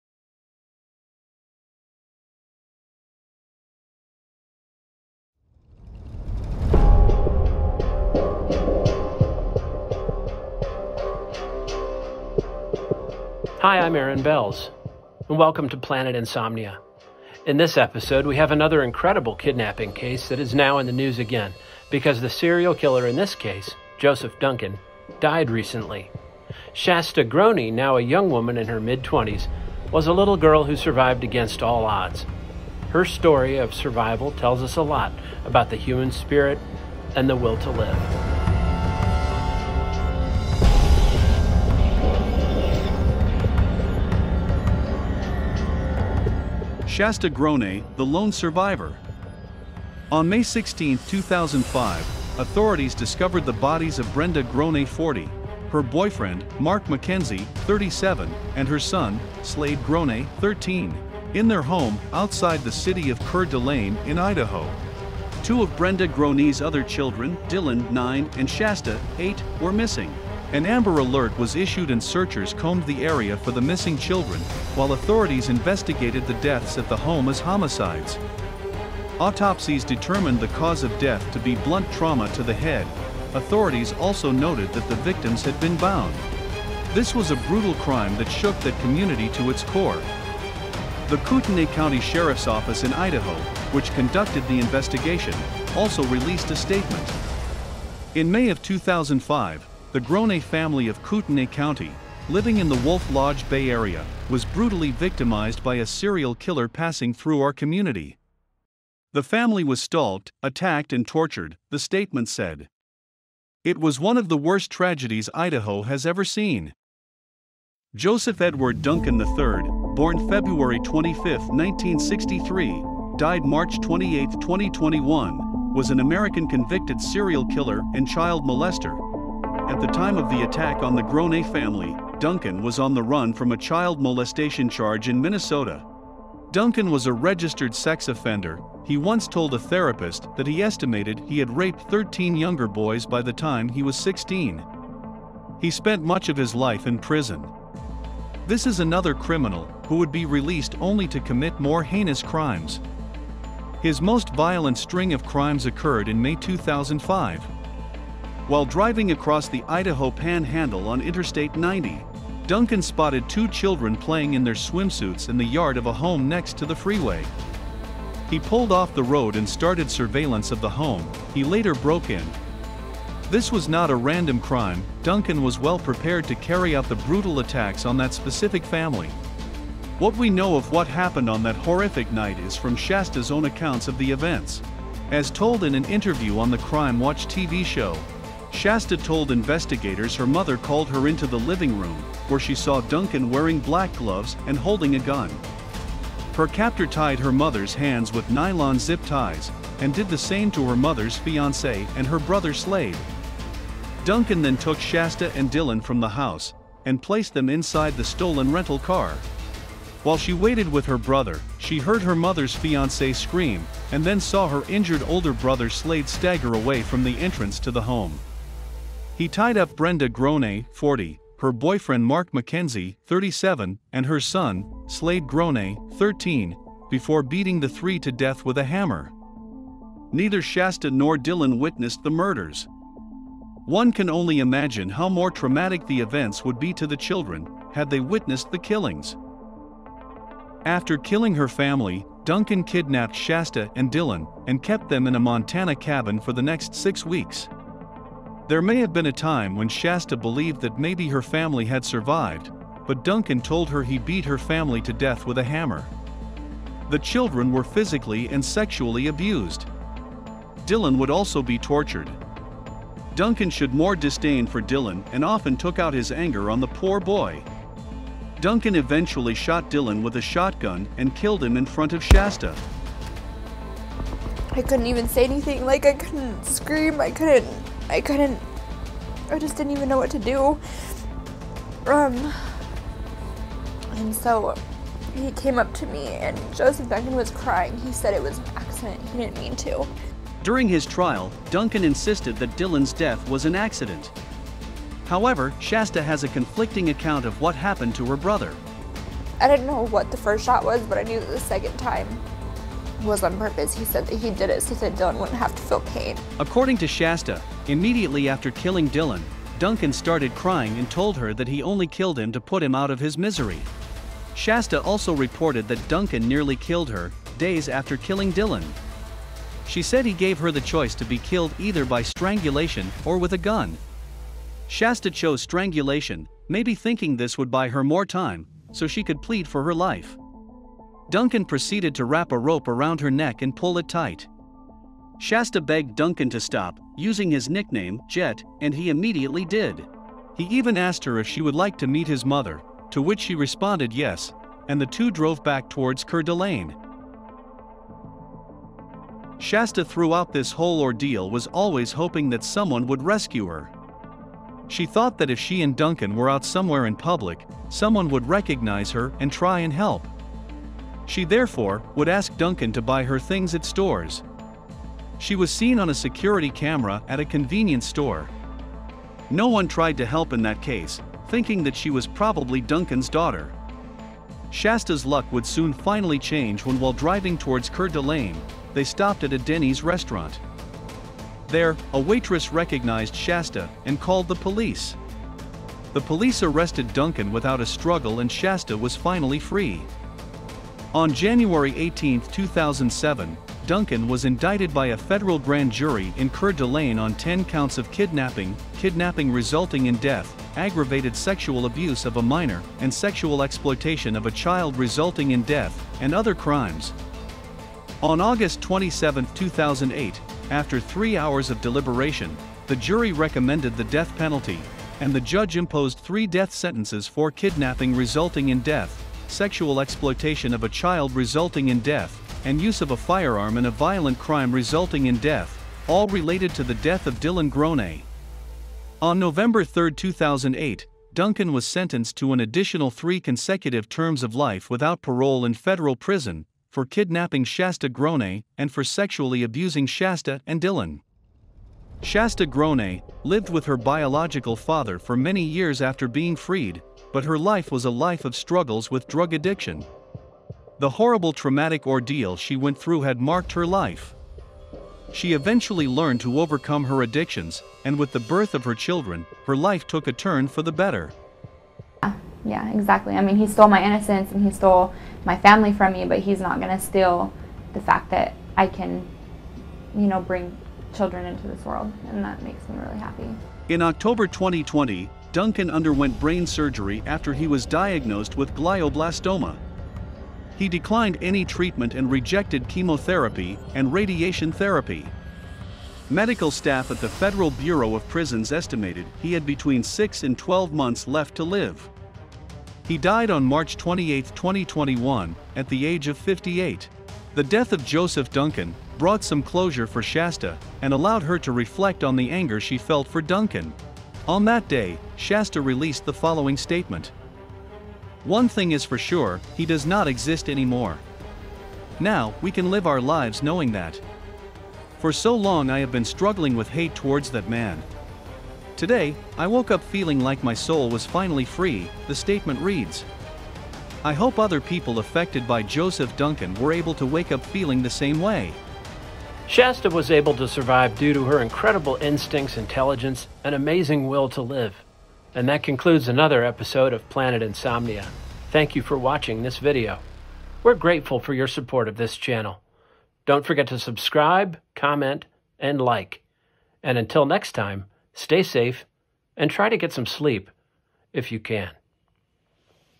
The Kidnapping of Shasta Groene - The Lone Survivor
In this episode of Planet Insomnia, we have another incredible kidnapping case that is now in the news again, because the serial killer in this case, Joseph Duncan,
died recently.

Shasta Groene, now a young woman in her mid twenties, was a little girl who survived against all odds. Her story of survival tells us a lot
about the human spirit and the will to survive.

On May 16, 2005, authorities discovered the bodies of Brenda Groene, 40; her boyfriend, Mark McKenzie, 37; and her son, Slade Groene, 13,
in their home along Lake Coeur d'Alene, outside the city of Coeur d'Alene, in Idaho.

Two of Brenda Groene's other children, Dylan, 9,and Shasta,
8, were missing.

An AMBER Alert was issued and searchers combed the area for the missing children while authorities investigated the deaths at the
home as homicides. Autopsies determined the cause of death to be "blunt trauma to the head";
authorities also noted that the victims had been bound.

This was a brutal crime that shook that community to its core.

The Kootenai County Sheriff´s Office in Idaho, which conducted the investigation, also released a statement.

'In May of 2005, the Groene Family of Kootenai County, living in the Wolf Lodge Bay area, was brutally victimized by a serial killer passing through our community. The family was stalked, attacked and tortured,' the statement said.

'It was one of the worst tragedies Idaho has ever seen.'

Joseph Edward Duncan the third,  (born February 25, 1963, died March 28, 2021),
was an American convicted serial killer and child molester, at the time of the attack on the Gronee family,
Duncan was on the run from a child molestation charge in Minnesota.